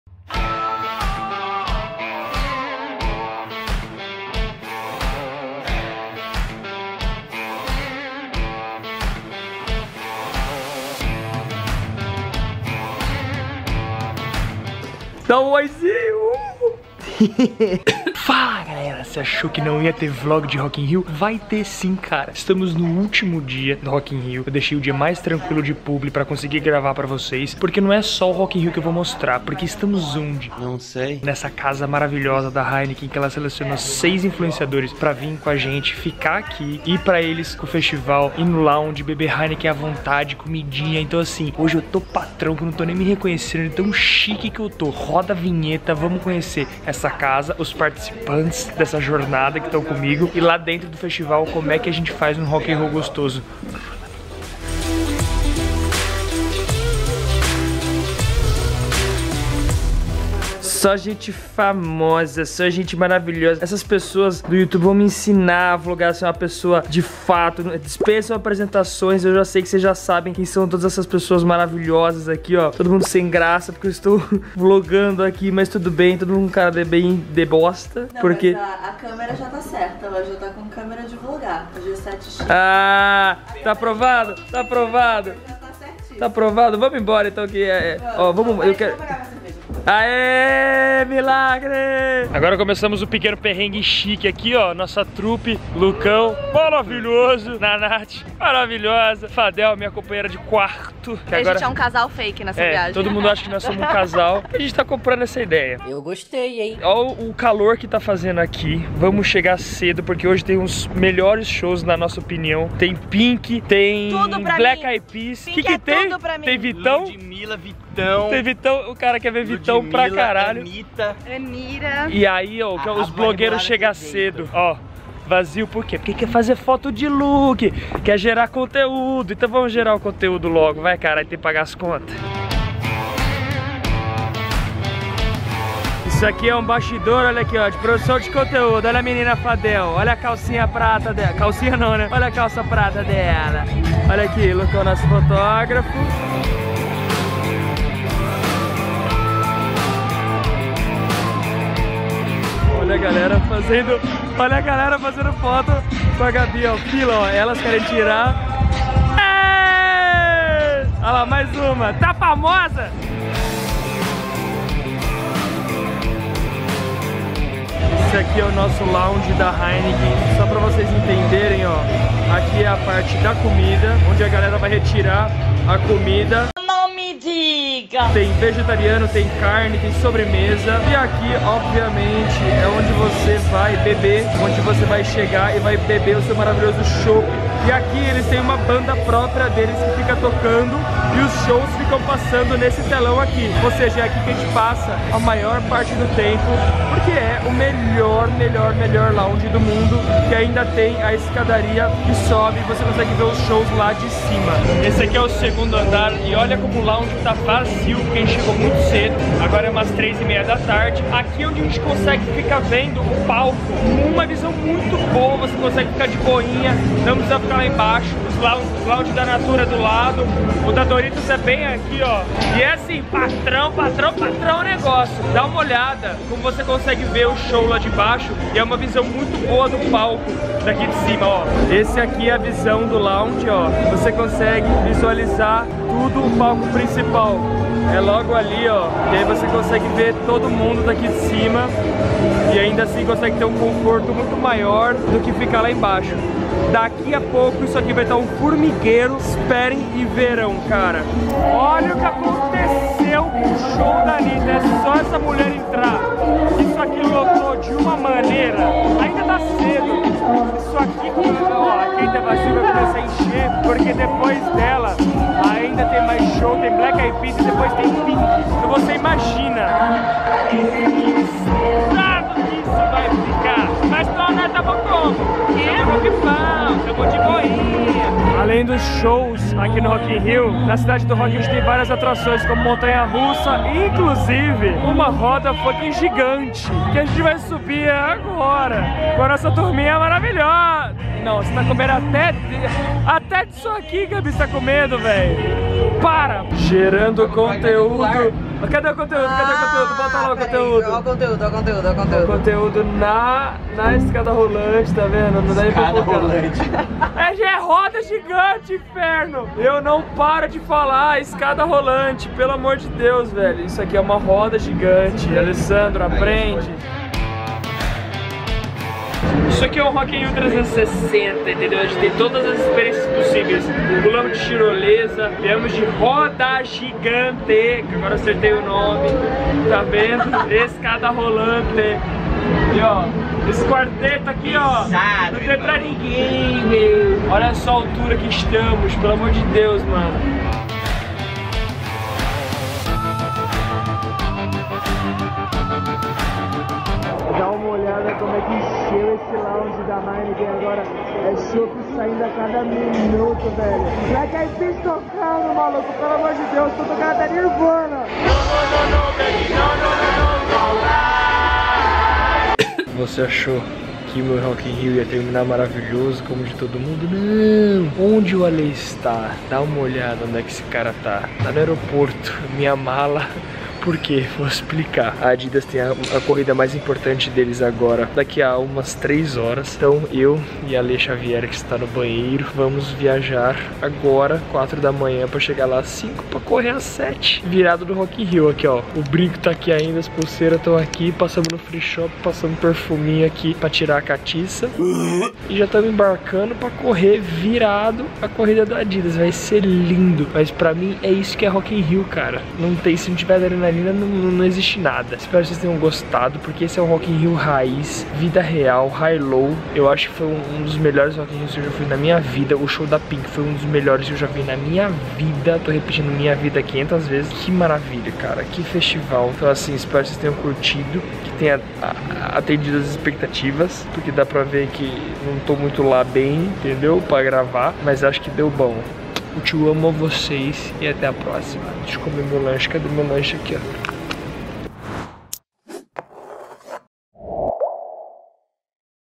ROCK IN RIO 2019 - SHOWS BONS? LOTADO? MUITO CALOR?
Mais um ano de RIR e sera que esse ano foi melhor que 2017?
Na minha opinião? SIM.
Shows melhores, infra estrutura melhor, mais qualidade no geral.
O clima foi cagado (no primeiro fds MUITA chuva e no segundo MUITO calor), mas isso não atrapalhou muito nossa felicidade em ver as bandas que gostamos.

Fiquei o segundo fds todo na casa da Heineken com mais outros 5 influenciadores, tivemos a experiência 360 com eles e foi SENSACIONAL.
Tentei mostrar como foi todo esse rolê e espero que vocês curtam também.

O tio ama vocês